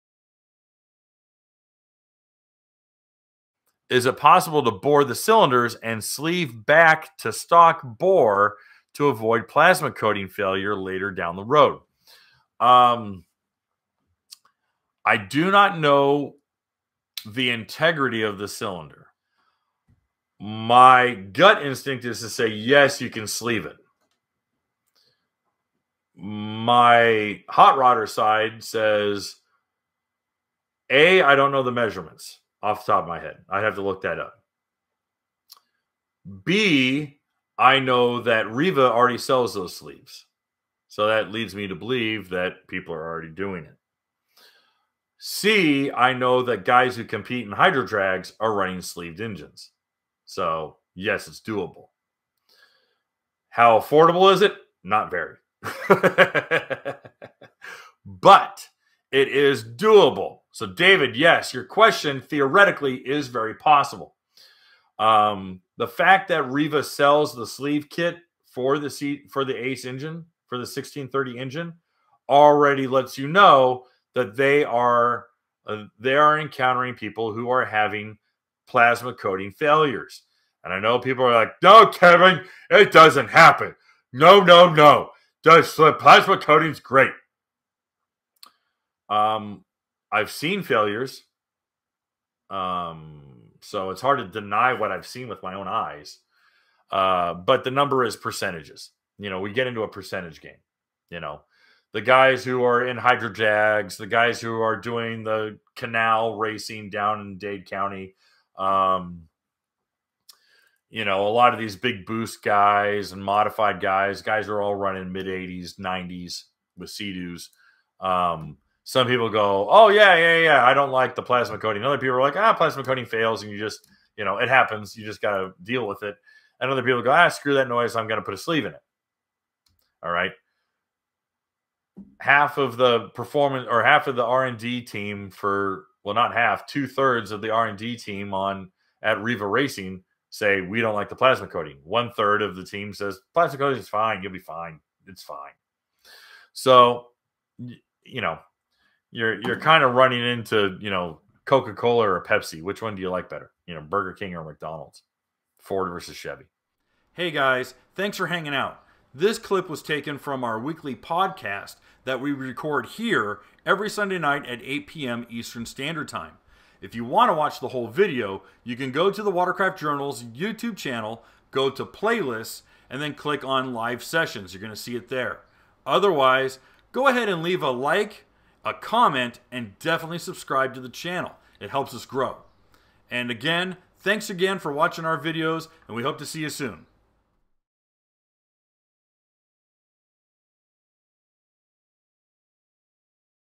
Can Sea-Doo's 1630 ACE Engine Be Fitted With Steel Sleeves? WCJ Quick Topic
This Quick Topic segment was taken from our full-length podcast titled "Realistic Near Future Watercraft Predictions" and can be viewed in its entirety